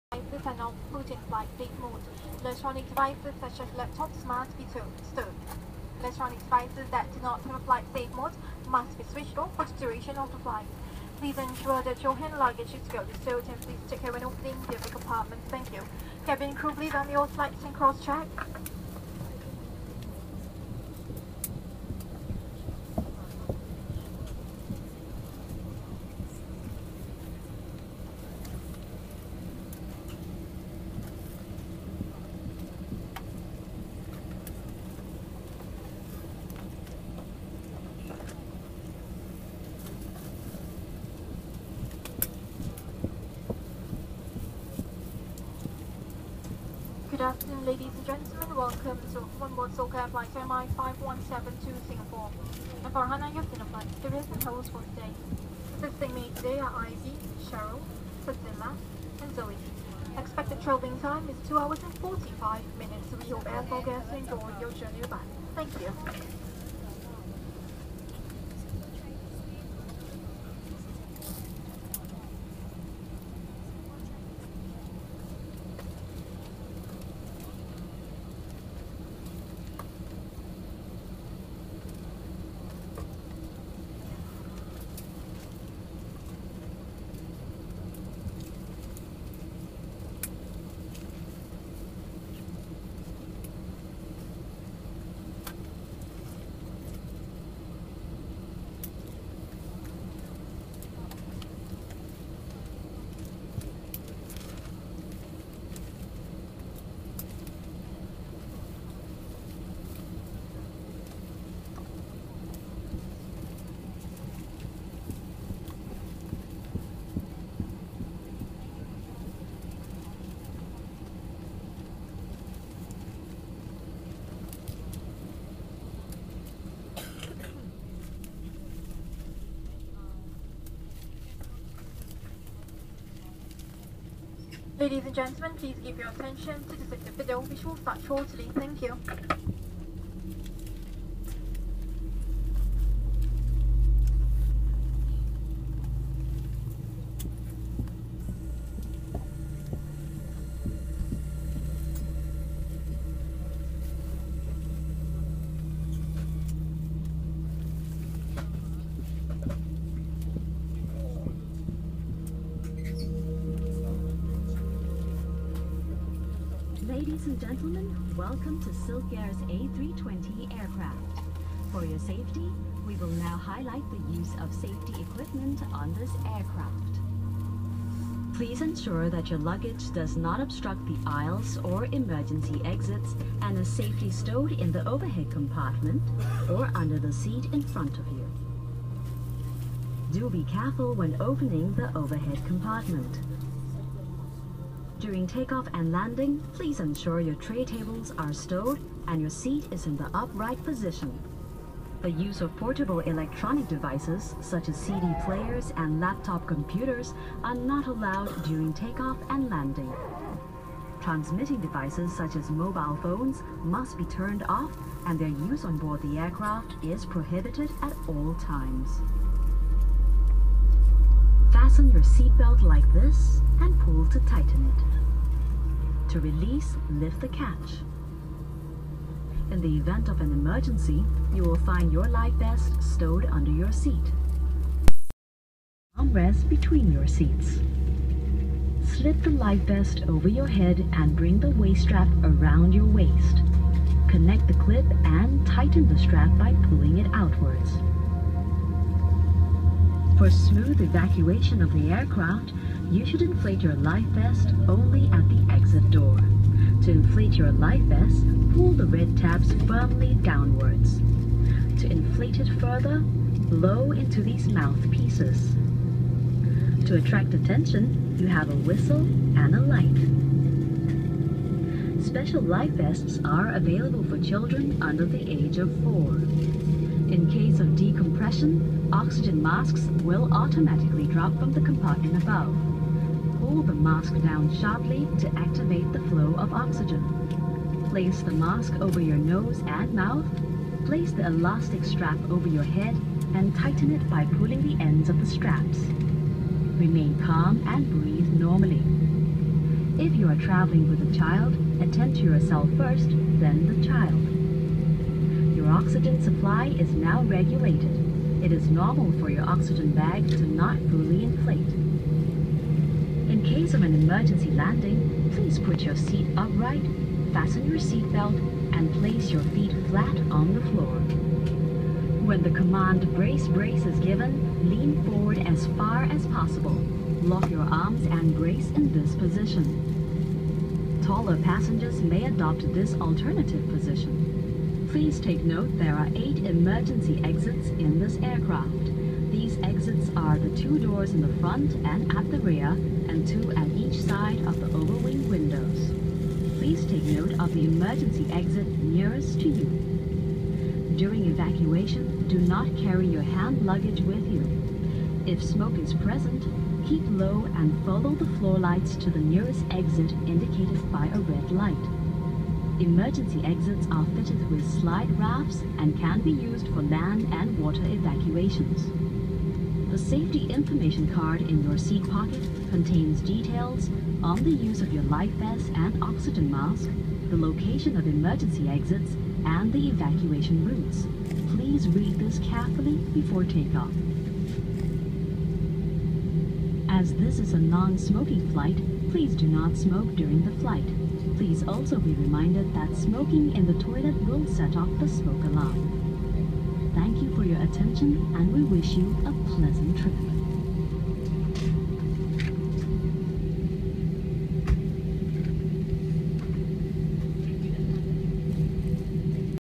Push back for departure from Yangon Airport
Myanmar people are very friendly, this ground grew does a thumbs up when he saw me filming him, I thumbs up back at him and he laugh!  lol.